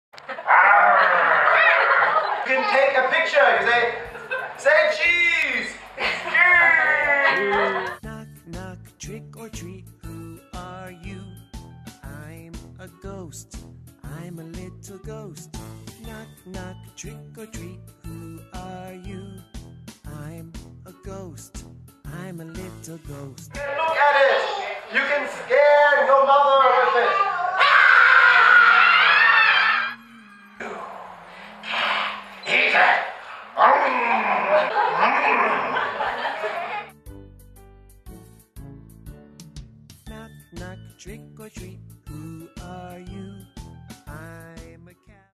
萬聖節 Halloween 點心-用蘋果.棉花糖.花生醬做「怪物嘴」 -《伊諾起思》
🎃跟著Tr. Mark伊起做點心Monster mouth吧！
✅來自New Zealand的Tr. Mark創意多點子妙，透過精彩故事繪本活動，帶給小朋友豐富有趣的故事饗宴：